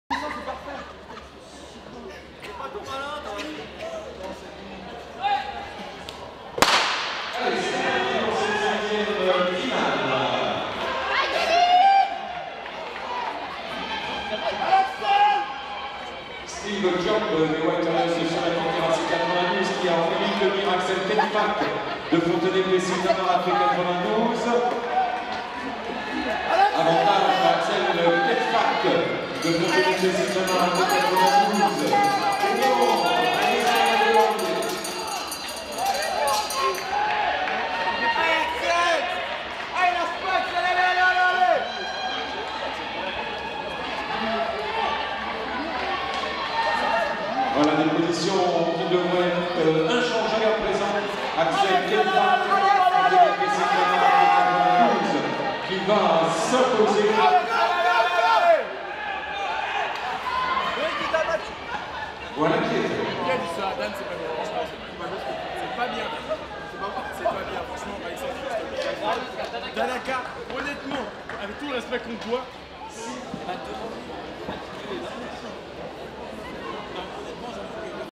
400m – Groupe B – Finale 5 – JESM – Championnat 92 & 78 Indoor 25/11/2017 – Eaubonne
400m - Salle / JESM-GB | M | F5 | Chr : E

1 - 52"52 - KENFACK Axel - Fpca92 - S/l Plessis Robinson
2 - 54"14 - MASSONI Jacques - Ac Cellois
3 - 54"78 - DJOB Steve - Whsnac 92 - S/l Nanterre Athl
4 - 55"13 - LORENZO Kevin - Fpca92 - S/l Csm Clamart
5 - 64"47 - DEFOSSE Georges - Athle78 - S/l Ua Maisons Laff